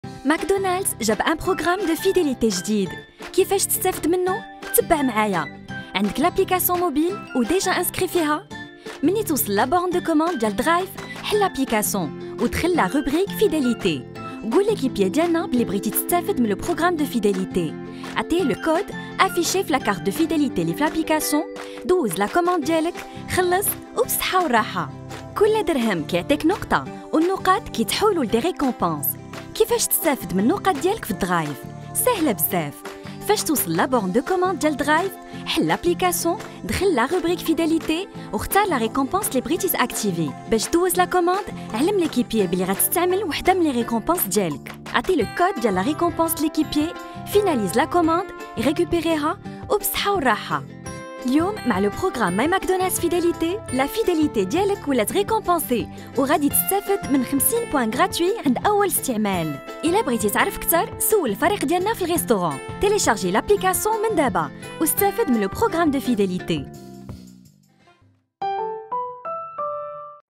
Programme myMcDonald's Fidélité - Au McDrive
Collectionne des points avec myMcDonald’s Fidélité ou utilise tes points pour des récompenses.
Voici comment ça marche au McDrive.